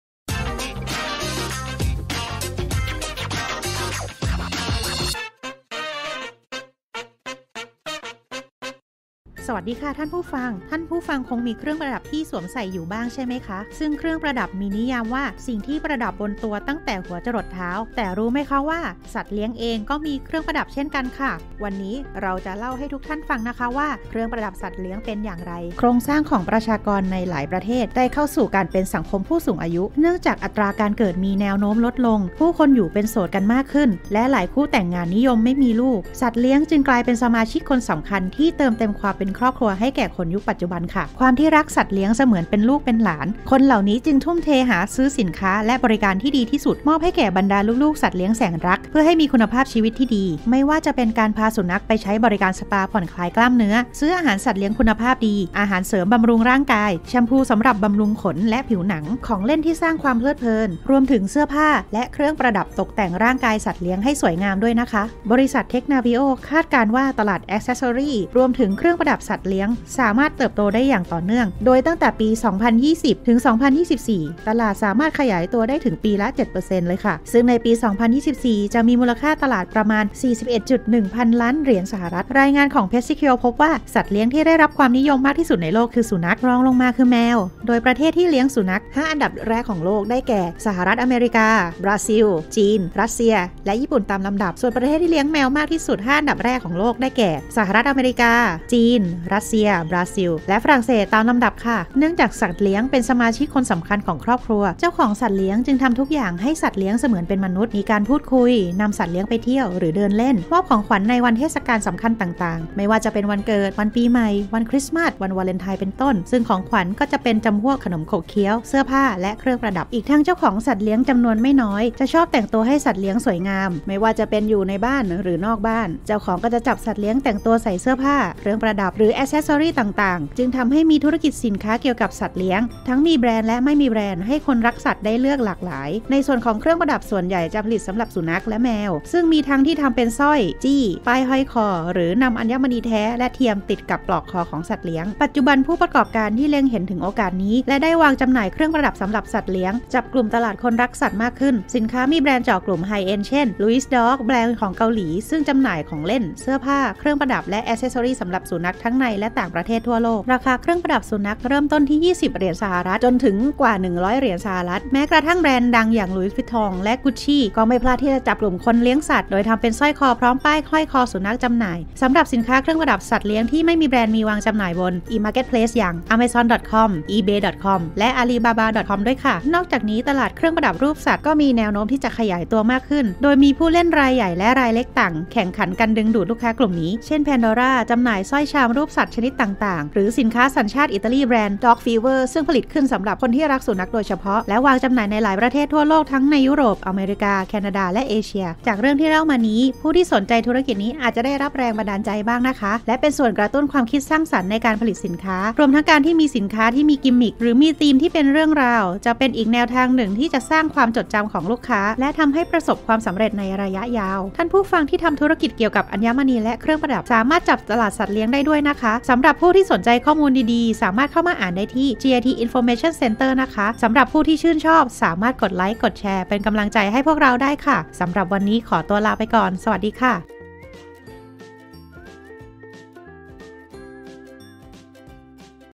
Story Behind Episode 7: เครื่องประดับสัตว์เลี้ยง
ในยุคปัจจุบัน มนุษย์ดูแลสัตว์เลี้ยงเหมือนกับสมาชิกในครอบครัว จนทำให้เกิดธุรกิจเครื่องประดับสำหรับสัตว์เลี้ยงที่ในปัจจุบันเป็นอีกธุรกิจที่น่าจับตามอง ใครอยู่ในธุรกิจนี้แล้วบ้าง? สามารถรับฟังได้ที่นี่